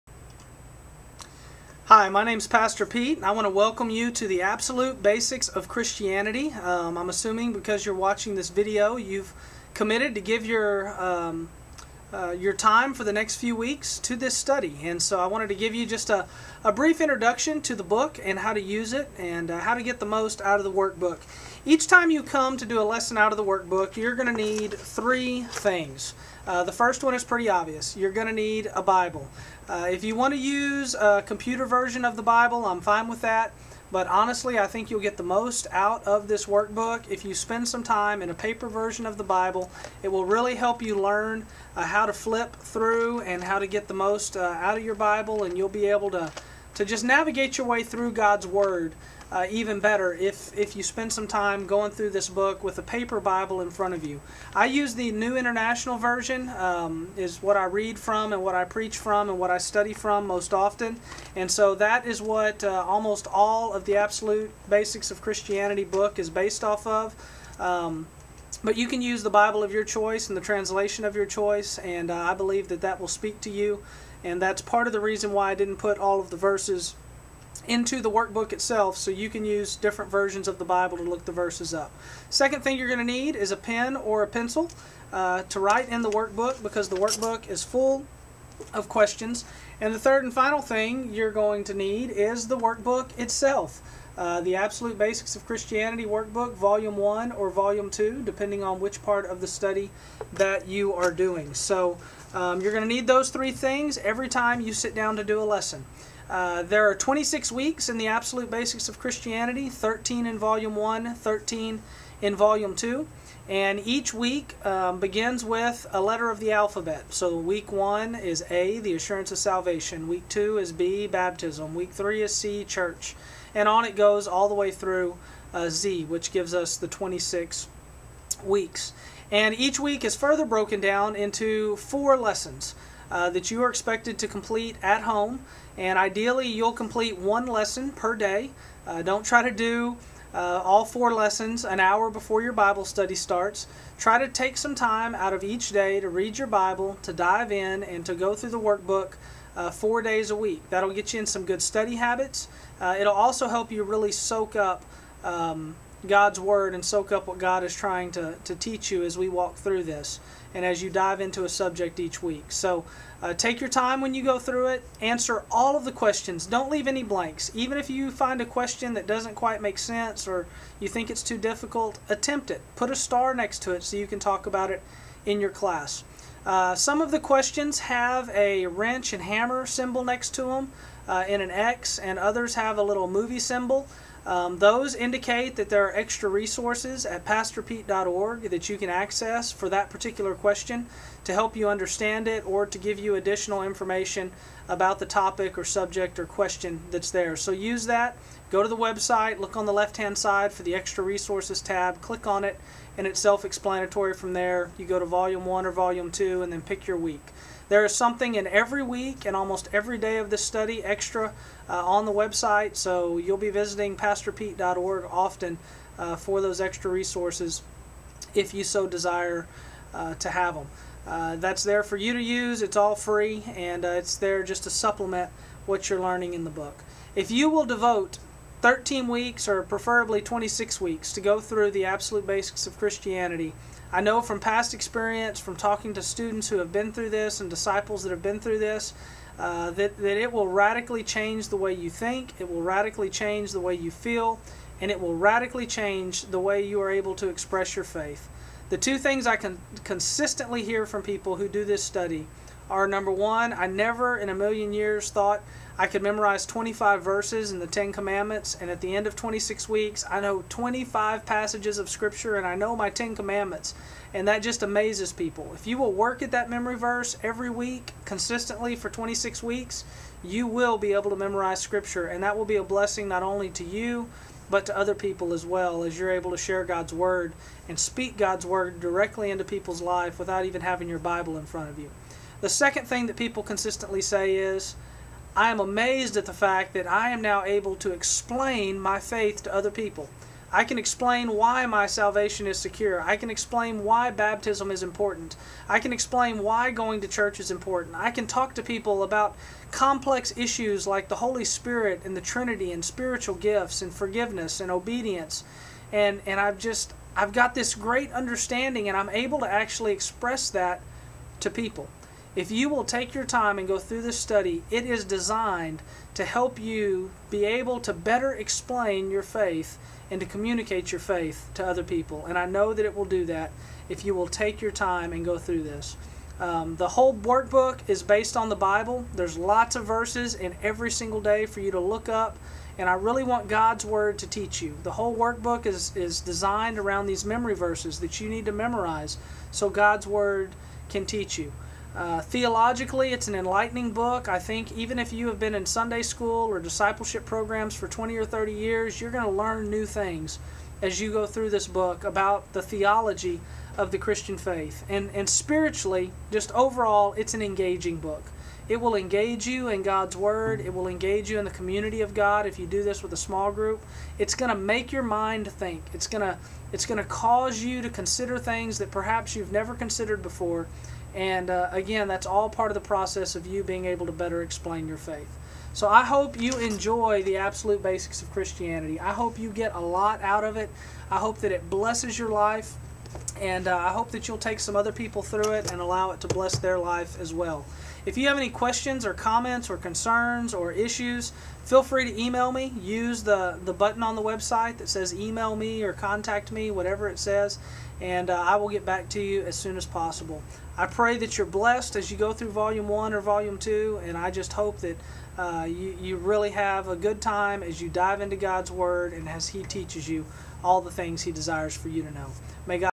The Absolute Basics of Christianity Vol. 1 Intro.mov
The Absolute Basics of Christianity is a Bible study workbook that takes a fresh look at the basics for all believers.  It is designed to help Christians better understand what they believe and be able to explain their beliefs to other.  To learn more about this two volume work by Pastor Pete Pawelek please